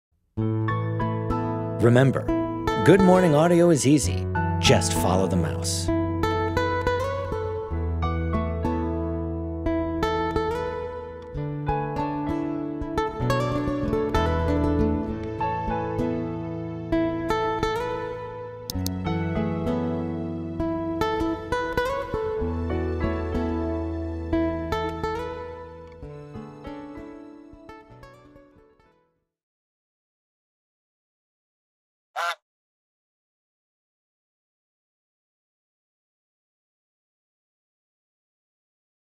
Pro Tools: Enabling Single Key Keyboard Shortcuts
Good Morning Audio!
This morning we learn how to enable keyboard shortcuts... its easy! Pro Tools allows you to "focus" your keyboard shortcuts into one of three different windows within the program. You can focus the keyboard onto the "clip list", the "groups" pane or within the "track view" or "edit window". For editing clips with single key shortcuts you'll want to click on the box labeled A/Z on the top right hand corner of the 'track view" view or "edit window".

The Good Morning Audio! guide:
Red Blips = left click of mouse
Green Blips = right click of the mouse
Keyboard Shortcuts are displayed along the bottom.

This video was created with Reaper 5.